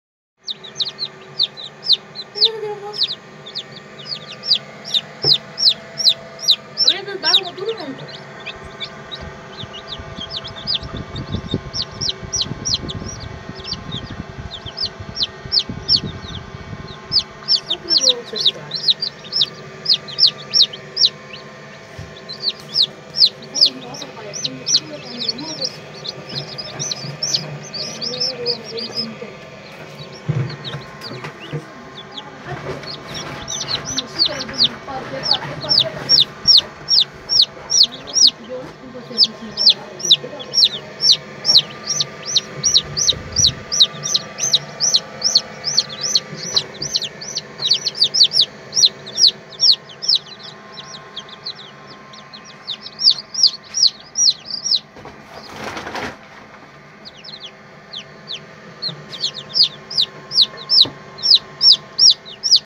CHOCADEIRA NASCENDO PINTAINHOS EM CABO VERDE - AFRICA FIGUEIRA DA NAUS DE HILARIO JOAO CANCIO LIMA
CHOCADEIRA DE CRIAÇÃO EM PAÍS CABO VERDE EM AFRICA